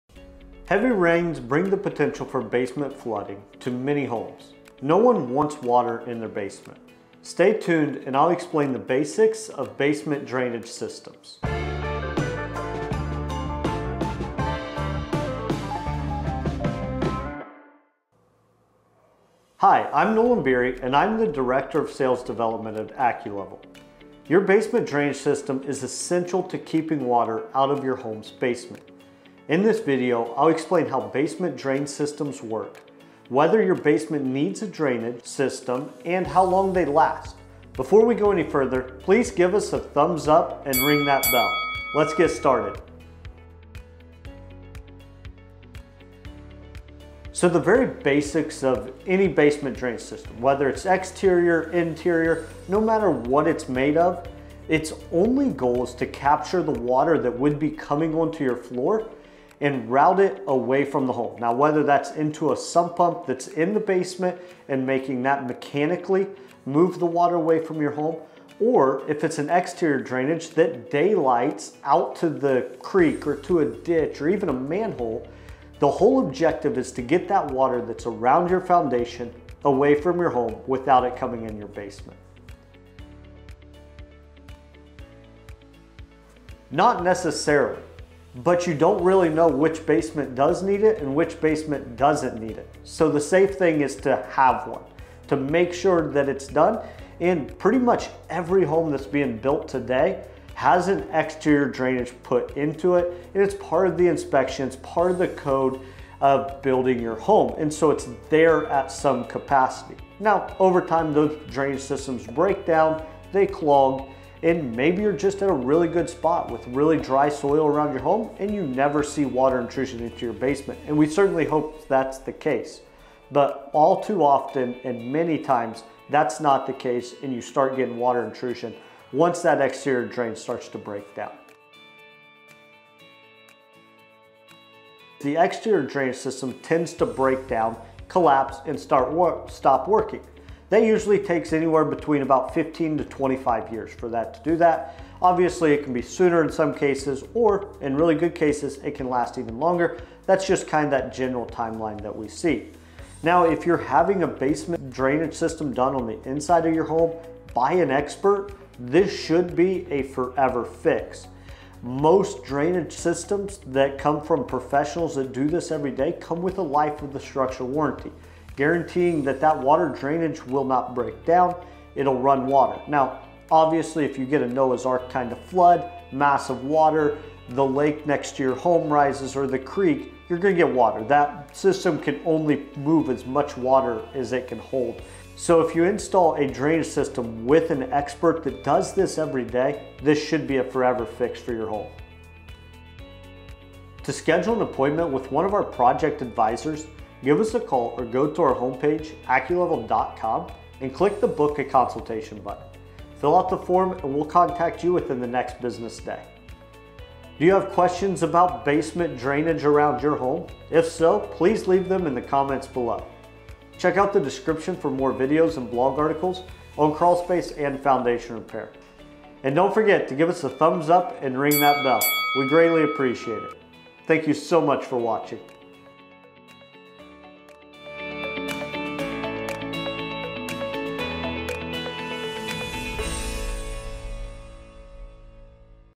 Basement Drainage Basics
The Basics of Basement Drainage.

Homes with basements usually have drainage installed. In this video, Nolan Beery explains the basics of basement drainage.

TIMESTAMPS
►0:00 Introduction
►0:44 How does a basement drainage system work?
►1:23 Does every home need a basement drainage system?
►2:12 How long do basement drainage systems last?
►3:12 Outro